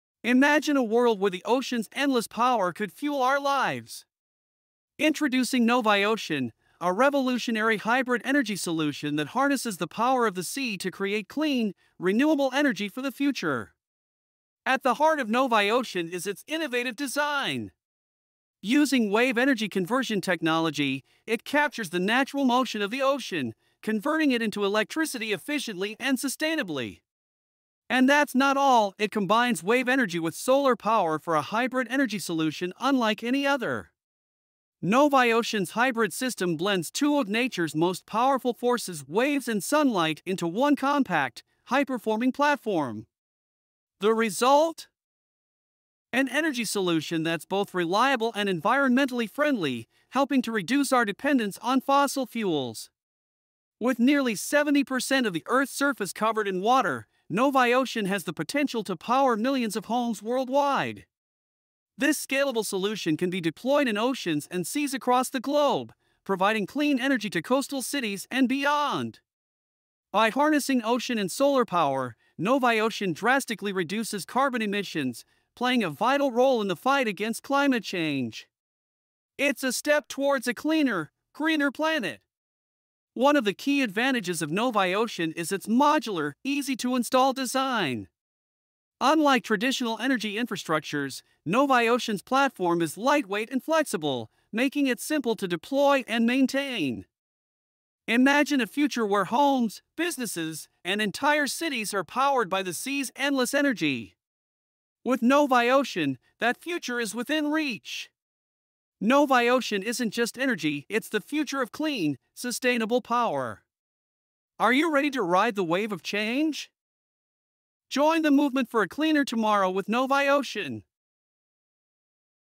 NoviOcean Hybrid Energy – The Future of Clean Power
NoviOcean is transforming how we generate clean energy, combining wave and solar power to create a hybrid energy system. Discover how this cutting-edge technology is harnessing the power of the ocean to power our homes and cities sustainably.